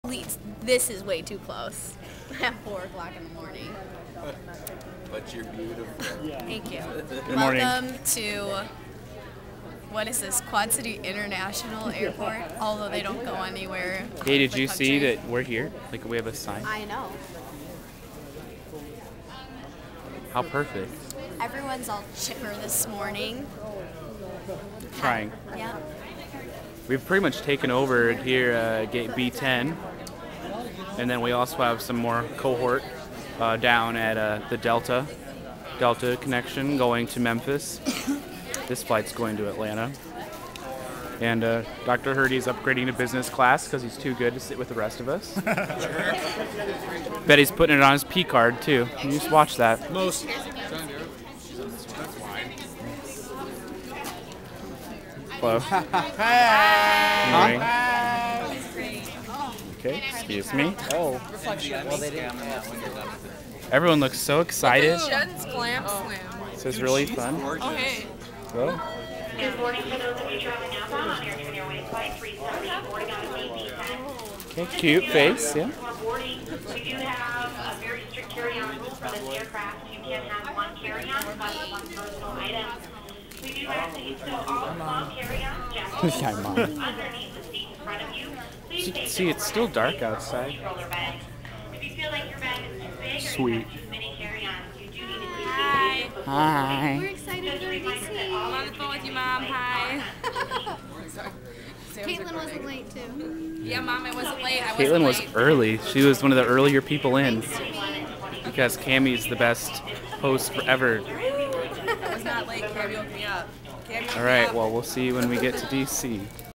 An Early Morning at MLI
The Choir waits to depart Moline for Washington, DC at 4:30am.  Yeah, it was early.